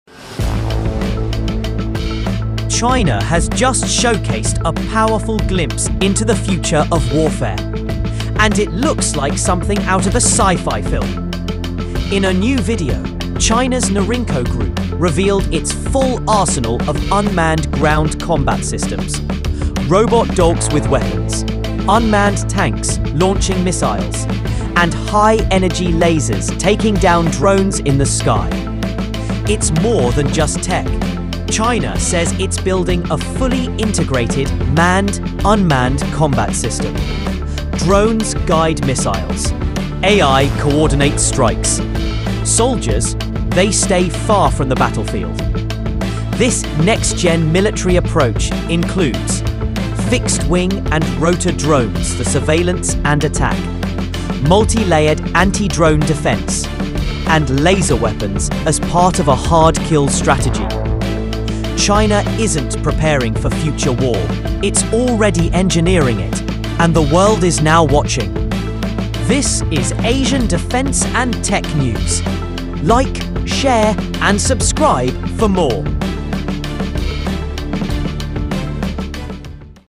“Robot Dogs and Laser Weapons: China’s War of Tomorrow”
China's future battlefield is already here. In this video, we explore the latest unmanned military technologies unveiled by China's Norinco Group — including robot dogs with weapons, unmanned missile-launching vehicles, drone-guided artillery, and high-energy laser weapons.

As China accelerates its military modernization, it's focusing on manned-unmanned integration, AI-driven combat systems, and multi-layered drone defense. From air defense missiles to cyber warfare, Norinco’s newly revealed video highlights how future wars may be fought with minimal human involvement.

🔴 Key Highlights:

Robot dogs with weapons

Unmanned tanks and missile systems

Anti-drone lasers and air defenses

AI-guided artillery and drones

Future warfare tech by Norinco Group

Watch till the end to see how China is combining robotics, artificial intelligence, and high-tech weaponry to reshape modern combat.